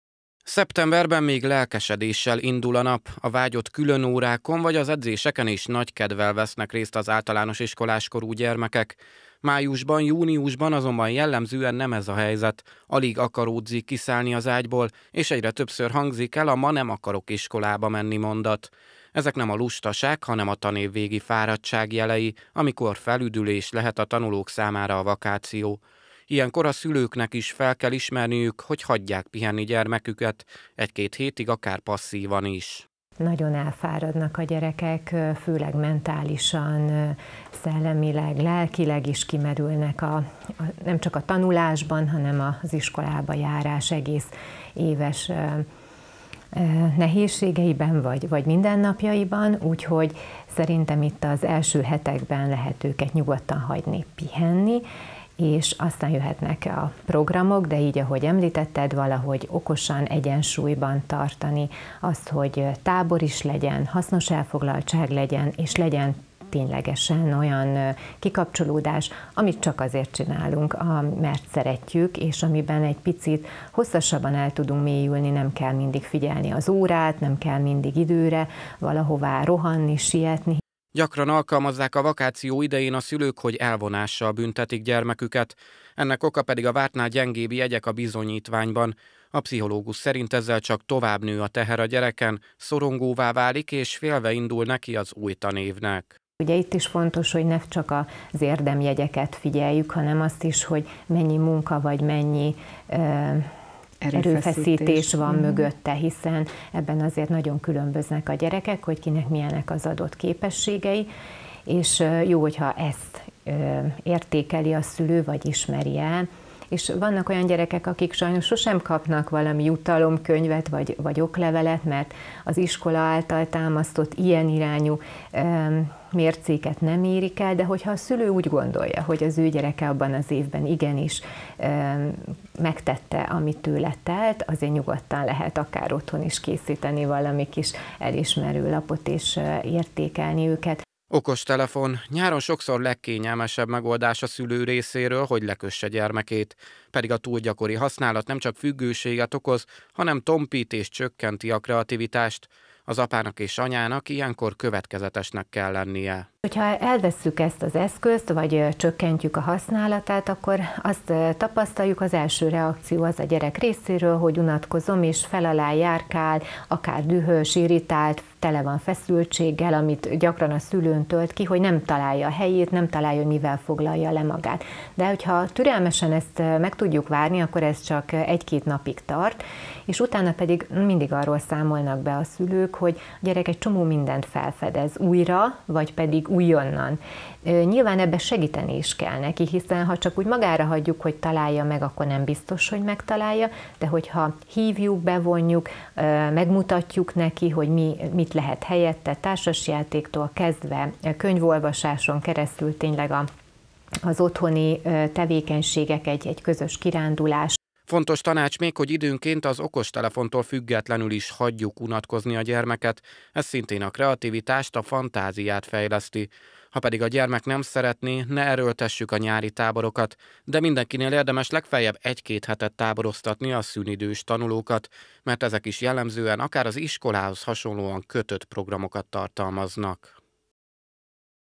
Így pihenjen a gyerek nyáron
Sok gyerek kimerült a tanév végére, ilyenkor sok múlik a szülőn is, hogy megfelelően kipihenjék magukat a vakáción.

Az általunk megkérdezett gyermekpszichológus szerint ilyenkor 1-2 hétig érdemes hagyni, hogy passzívan pihenjenek, vagyis többet lustálkodjanak. A szakember elmondta, sok szülő esik abba a hibába, hogy ilyenkor a rosszabb jegyek miatt büntetik gyermeküket. Ez ugyanakkor nem jó megoldás, de a jó teljesítményt ajánlott értékelni.